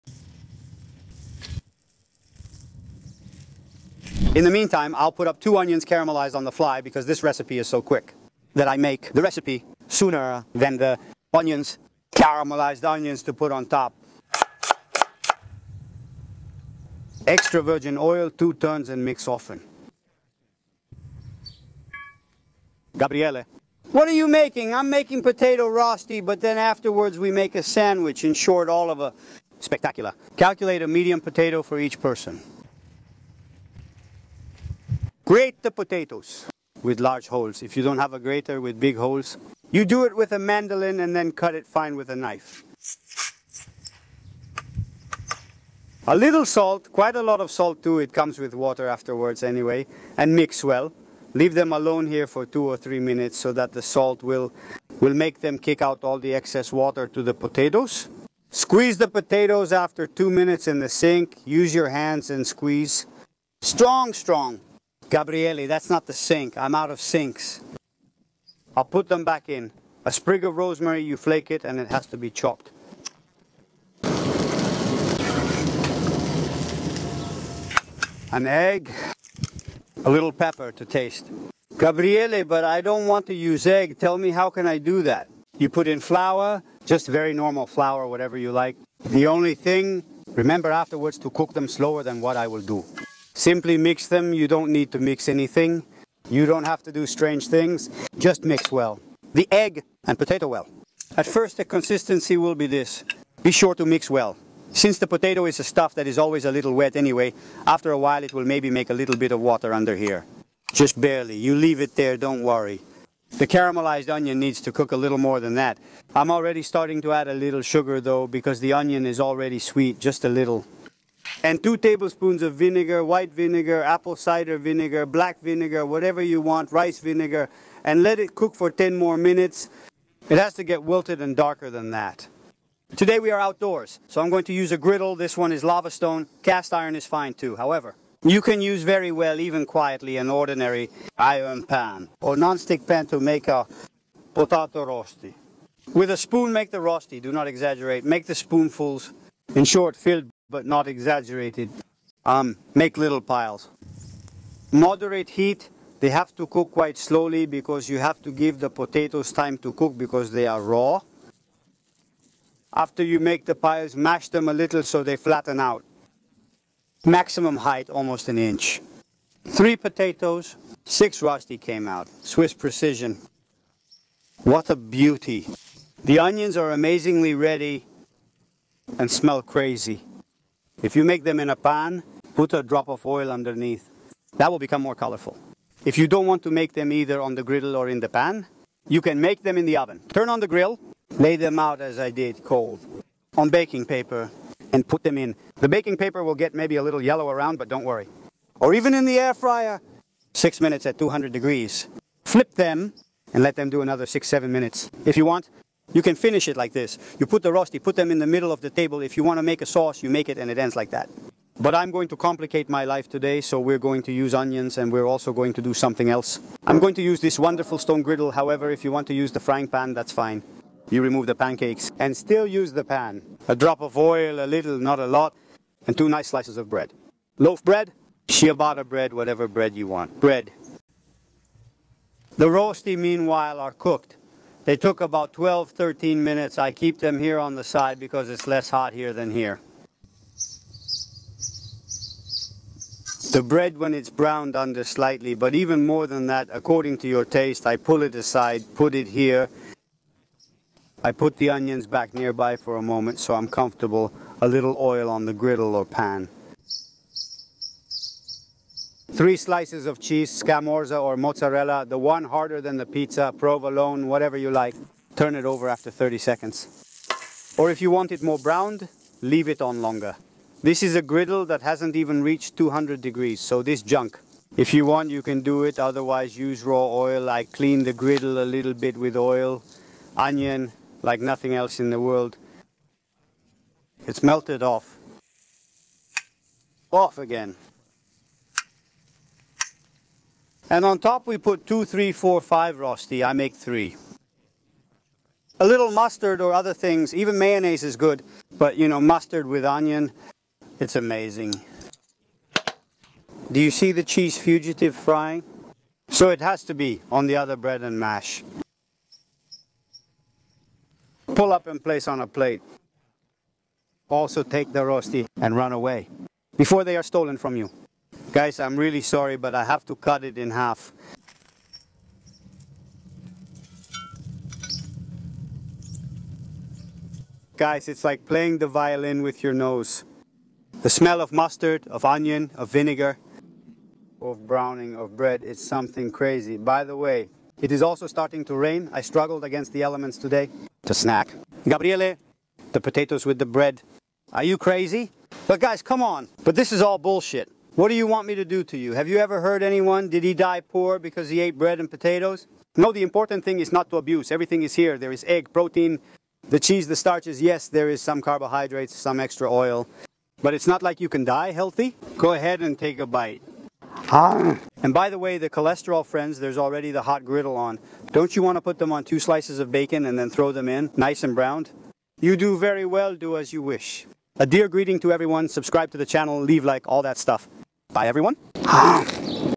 SANDWICH WITH POTATO ROSTI - Rustic sandwich with potato rosti and grilled cheese
If you stick to the cliché of never eating potatoes with bread, you'll miss this miracle of griddled potato rosti with caramelized onions and smoked scamorza cheese in a sandwich. But do you hear how good that sounds, or do you?

Ingredients:
3 potatoes
(1 medium potato for each person)
1 egg
Salt, pepper, oil to taste
Rosemary or parsley

To make onions
2 onions
½ tbsp sugar
3 tbsp vinegar

Smoked scamorza cheese
Bread
Mustard to taste

How to make it:
In a pan, add two turns of oil and the sliced onions.
Fry until the onions are browned.
Add salt and pepper, then sugar and vinegar.
Cook over medium heat for another 10 minutes or until the onions are well cooked and browned.
Grate the large potatoes or use a mandolin to make strips to cut with a knife later.
Salt the potatoes with a teaspoon of salt, mix well, wait 5 minutes.
The potatoes with the salt will make a lot of water, squeeze them well and put them in a bowl.

On a griddle or in a large, lightly oiled skillet, use a spoon to make mounds with the potatoes, then use the same spoon to mash the potatoes a little, creating burgers about 1 inch high.
Cook over a low heat until browned, then flip.
Cook for another 10 minutes.
In the same pan, make a nice bruschetta, cut it in half and melt 3 nice slices of scamorza cheese for each sandwich.

Add pepper, some chopped rosemary or parsley and an egg.

Assemble the sandwich when the bread is browned.